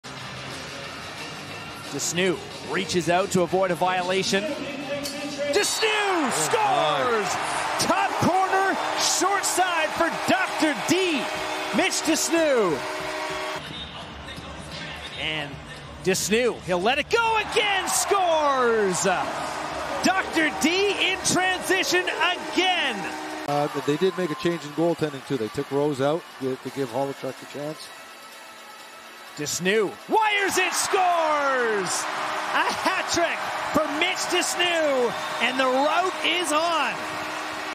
Mitch de Snoo gets the hat trick in Rock blowout win
Defender Mitch de Snoo scores three goals in transition for the Toronto Rock in a 17-8 win over Halifax. January 14, 2023.